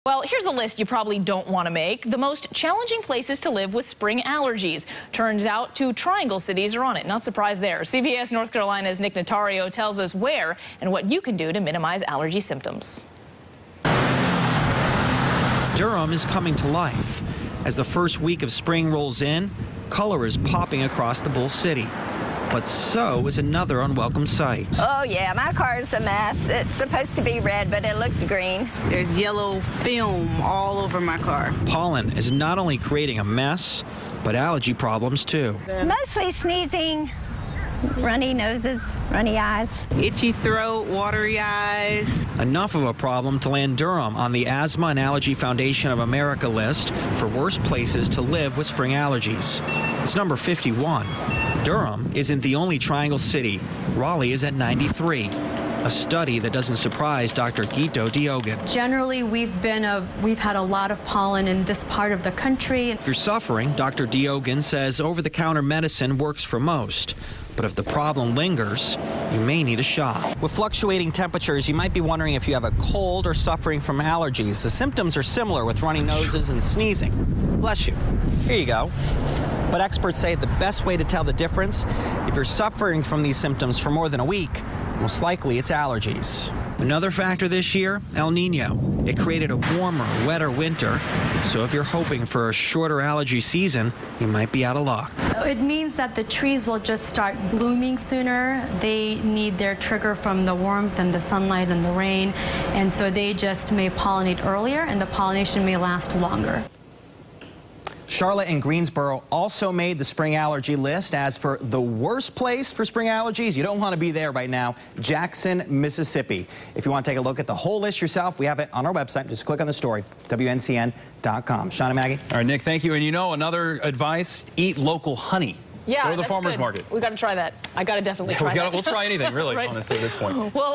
Raleigh, Durham make list for worst places to live with Spring allergies
Two Triangle cities made a list that some people probably want no part of.